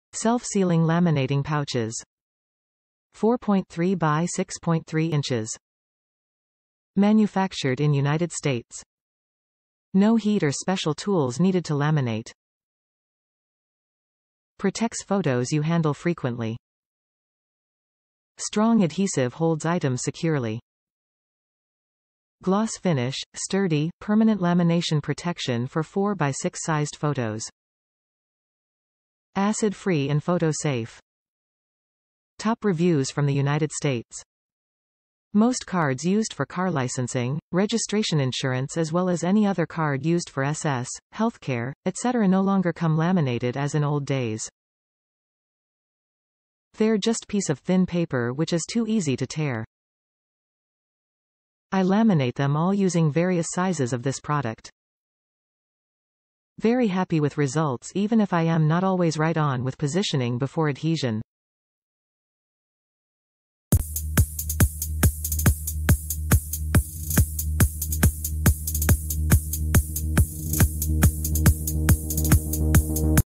Scotch Self-Sealing Laminating Pouches, Glossy Finish, 4.3 x 6.3 Inches, 5 Pouches
Self-sealing laminating pouches
4.3 x 6.3 Inches
Manufactured in united states
No Heat or special tools needed to Laminate
Protects photos you handle frequently
Strong Adhesive holds items securely
Gloss finish, sturdy, permanent lamination protection for 4x6 sized photos
Acid-free and photo-safe

Most cards used for car licensing, registration & insurance as well as any other card used for SS, health care, etc no longer come laminated as in old days. They're just piece of thin paper which is too easy to tear. I laminate them all using various sizes of this product. Very happy with results even if I'm not always right on with positioning before adhesion.